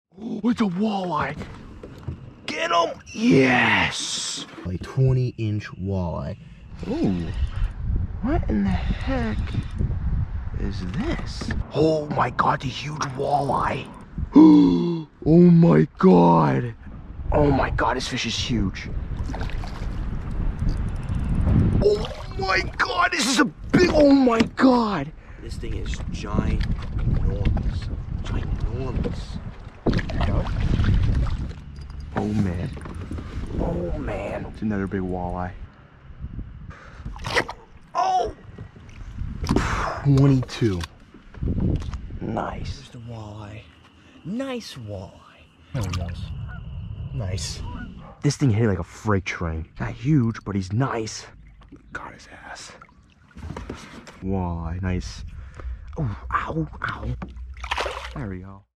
Great Walleye Catches on the Kayak! ~ Walleye Kayak Fishing ~
In this short, I collected all of my great walleye catches on the kayak from last spring. These were great days fishing for bass, walleye and pike on the kayak. These were some good size walleye, especially my biggest walleye that probably weighed about 8 pounds.